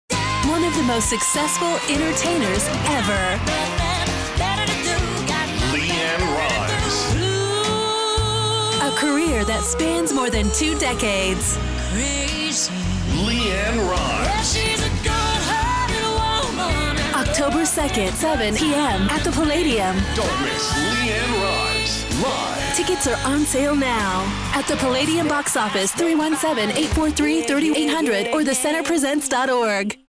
LeAnn Rimes Oct. 2 | at the Palladium
LeAnn Rimes is one of the most successful entertainers with a career spanning more than two decades. With 44 million albums sold, two Grammy Awards, three Academy of Country Music Awards and 12 Billboard Music Awards, she has made her mark in the world of music. Unstoppable from the age of 11, LeAnn was the youngest person ever to win a Grammy Award and was the first country artist to win a Grammy for Best New Artist.

See LeAnn Rimes live October 2, 2016 at 7pm.